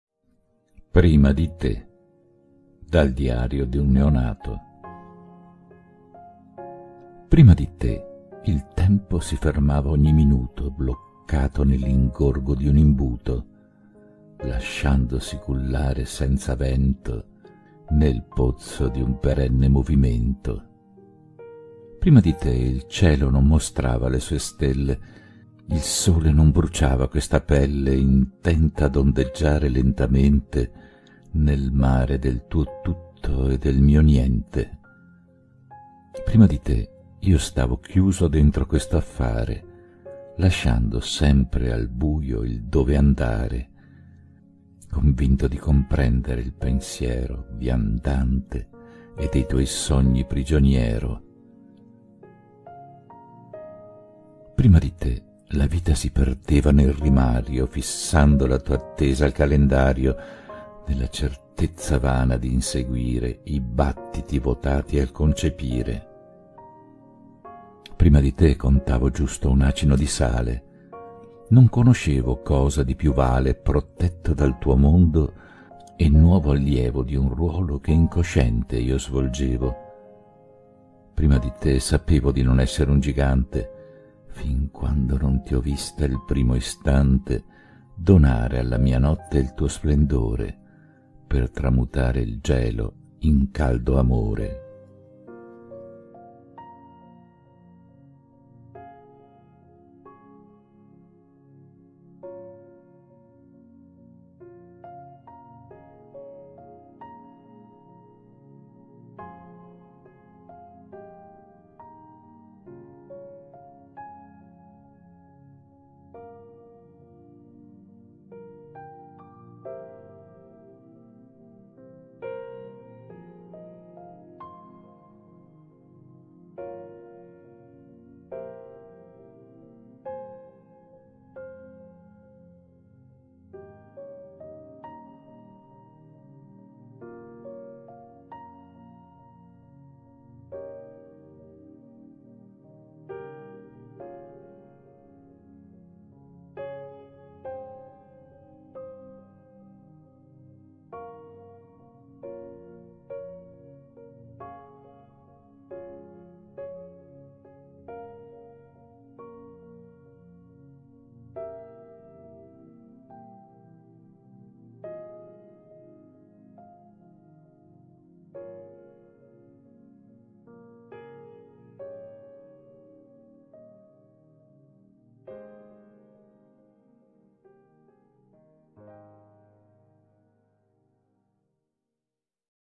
Prima di te (Dal diario di un neonato) byPeppeCassese - video di Anna Parascandolo
Prima di te (Dal diario di un neonato) byPeppeCassese - video di Anna Parascandolo - Legge Rodolfo Vettor

Prima di te
il tempo si fermava ogni minuto
bloccato nell’ingorgo di un imbuto
lasciandosi cullare senza vento
nel pozzo di un perenne movimento.

Prima di te
il cielo non mostrava le sue stelle
il sole non bruciava questa pelle
intenta ad ondeggiare lentamente
nel mare del tuo tutto e del mio niente.

Prima di te
io stavo chiuso dentro questo affare
lasciando sempre al buio il dove andare
convinto di comprendere il pensiero
viandante e dei tuoi sogni prigioniero.

Prima di te
la vita si perdeva nel rimario
fissando la tua attesa al calendario
nella certezza vana di inseguire
i battiti votati al concepire.

Prima di te
contavo giusto un acino di sale
non conoscevo cosa di più vale
protetto dal tuo mondo e nuovo allievo
di un ruolo che incosciente io svolgevo.

Prima di te
sapevo di non essere un gigante
fin quando non ti ho vista il primo istante
donare alla mia notte il tuo splendore
per tramutare il gelo in caldo amore.

Musica
Romantico pianoforte
Creative video
annare53 💕